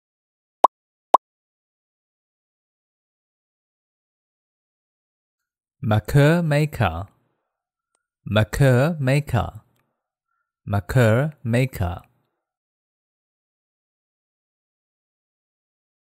How to Pronounce Makur Maker
In this video we'll show you how to correctly pronounce:  Makur Maker.

This channel is dedicated to improving your English Pronunciation through English Jokes, Tongue Twisters &Trending Words,